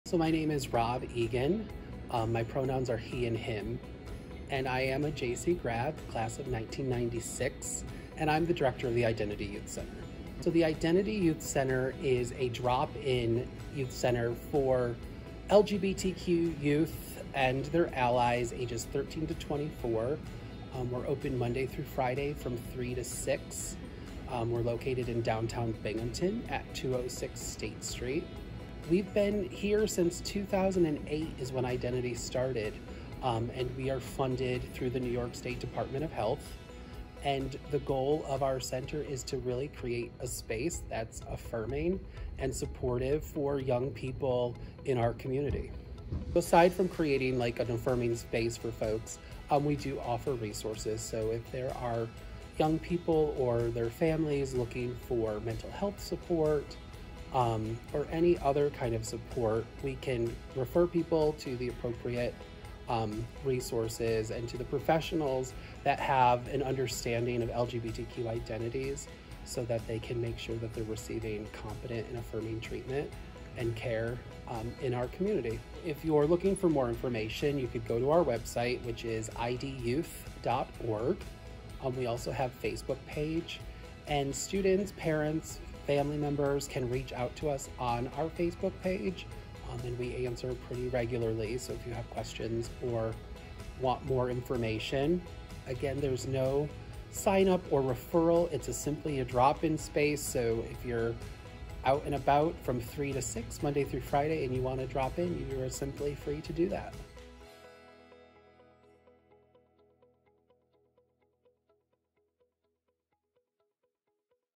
Spotlight On: Identity LGBTQ+ Youth Center
JC Connects spotlights community partner Identity LGBTQ+ Youth Center.